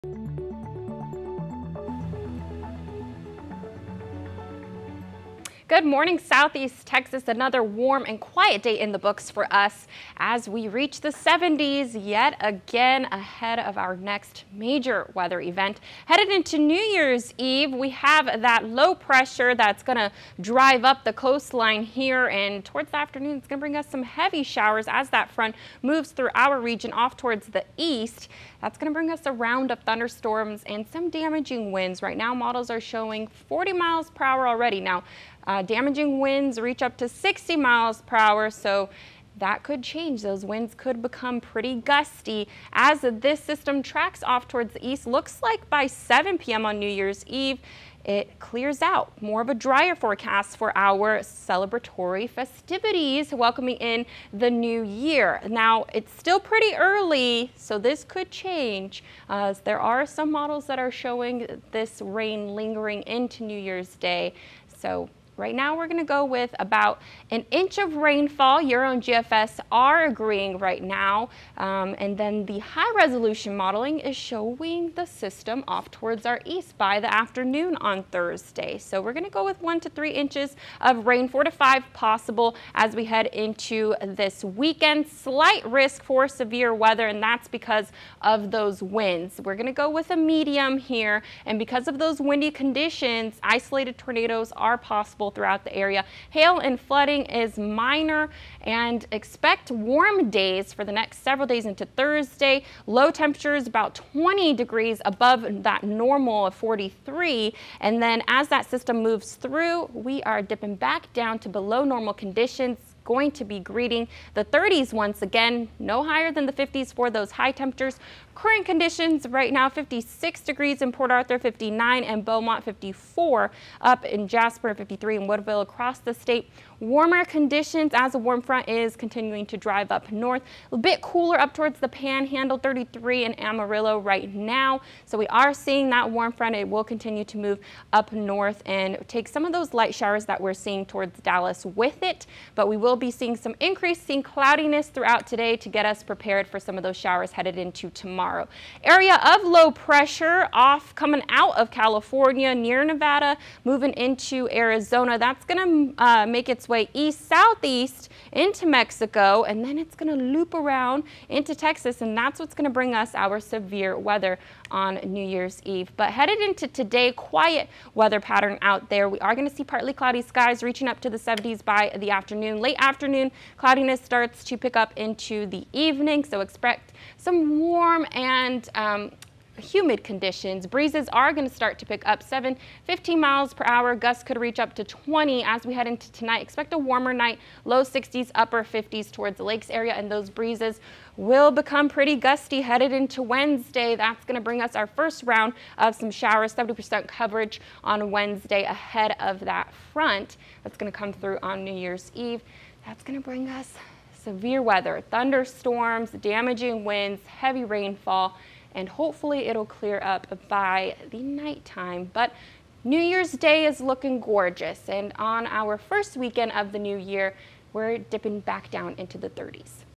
Warm front moving northward ahead of wet, cloudy Wednesday
Gusty winds and cooler temperatures are on the forecast for New Year's Eve.